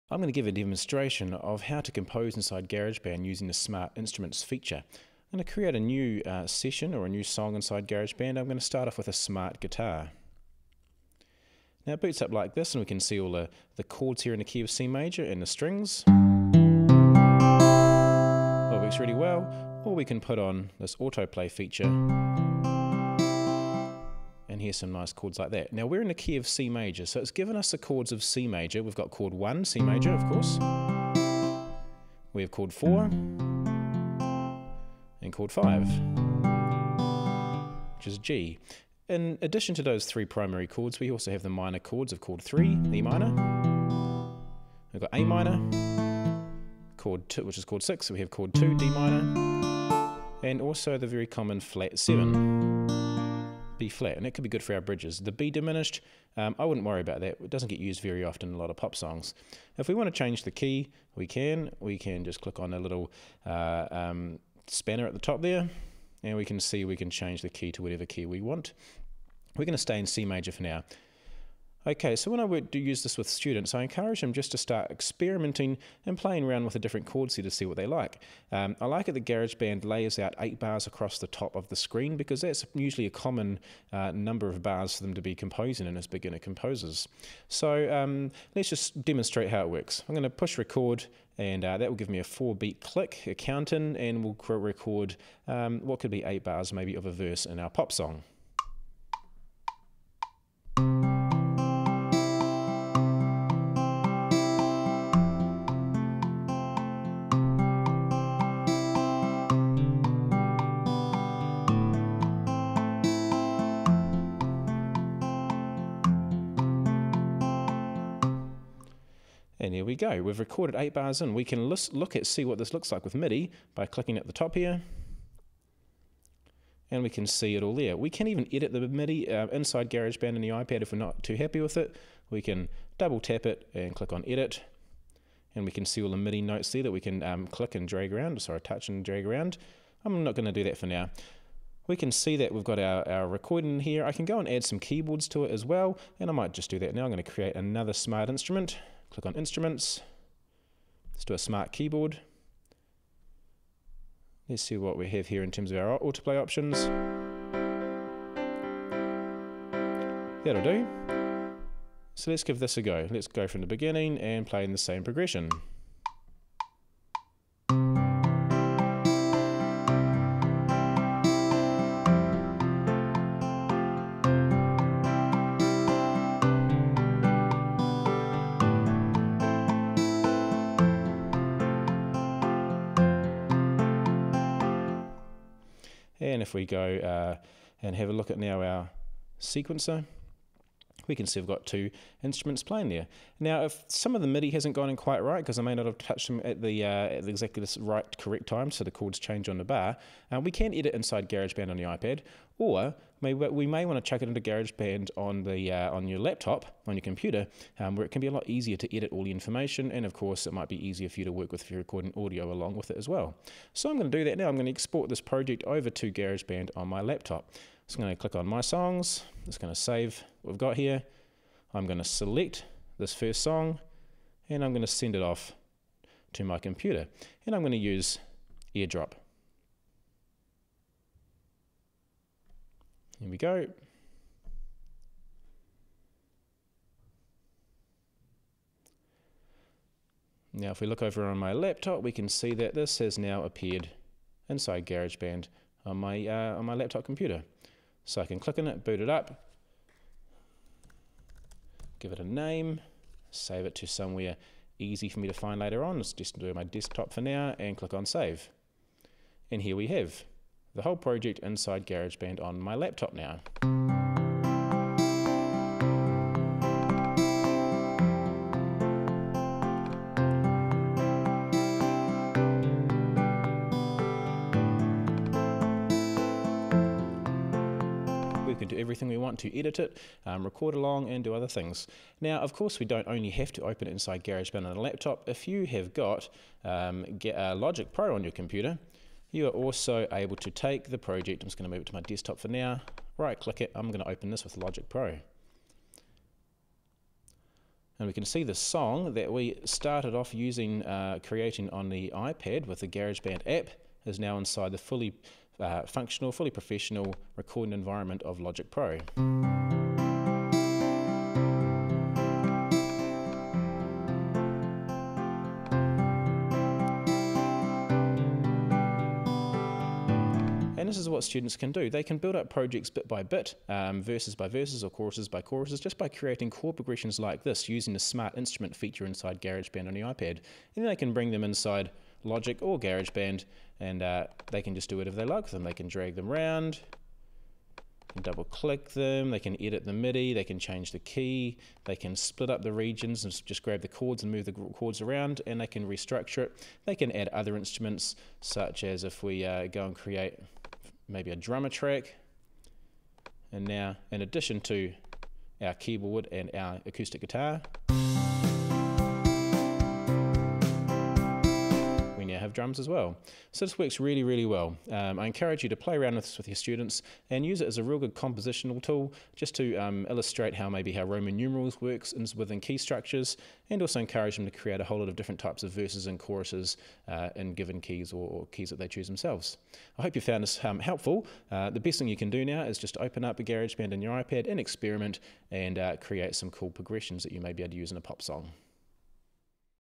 Using Garageband on iPad for creating chord progressions
Showing how easy it is to create chord progressions for songs using Garageband on the iPad.